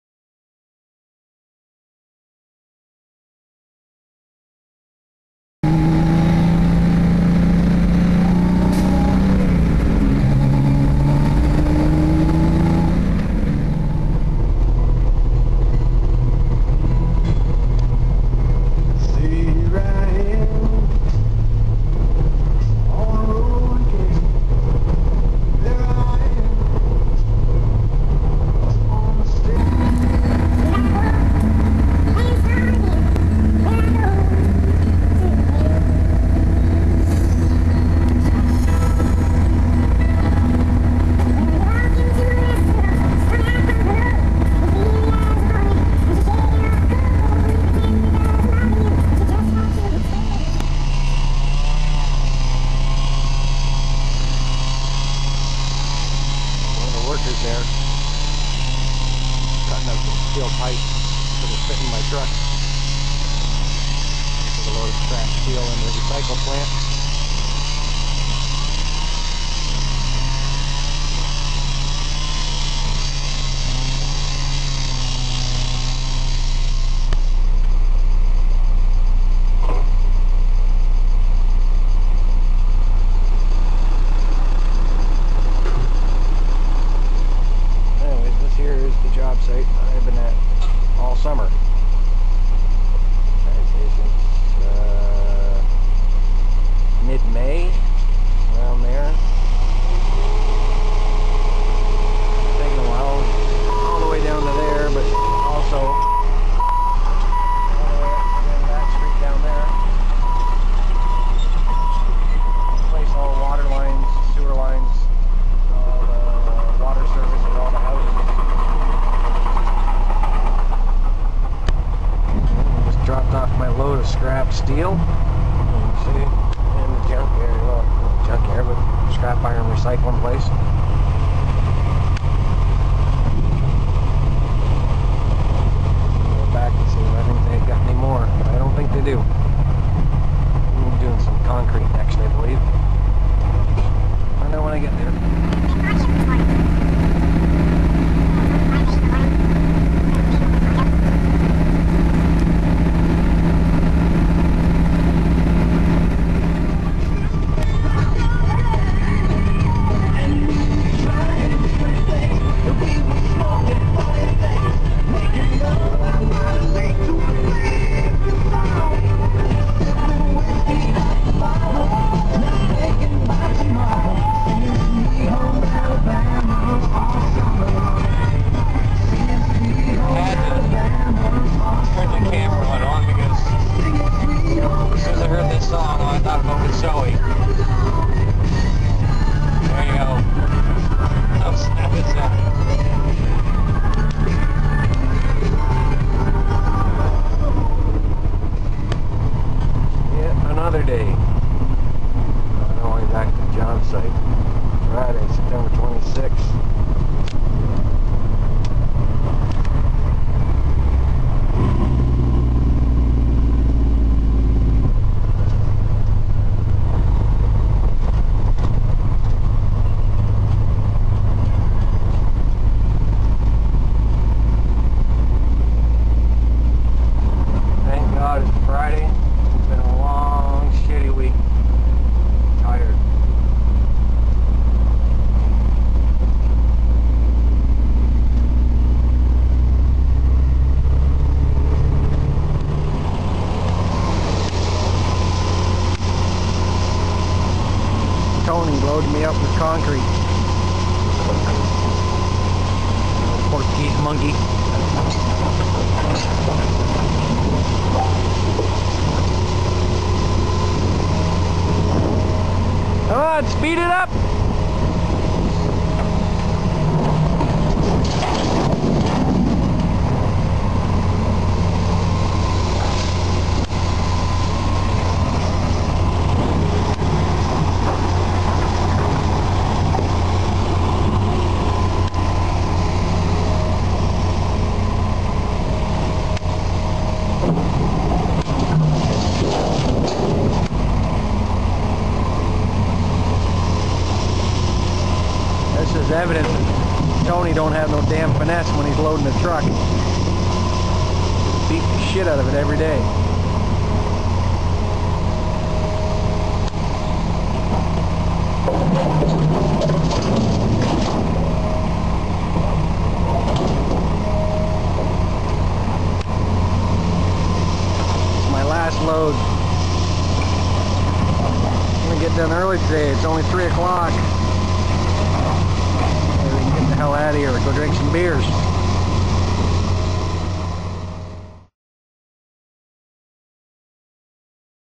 Last Couple Of Days Trucking
Just some clips of the last couple of days leading up to Friday...BEER DAY!!! Wooooooooo Hoooooooo...Cheers!!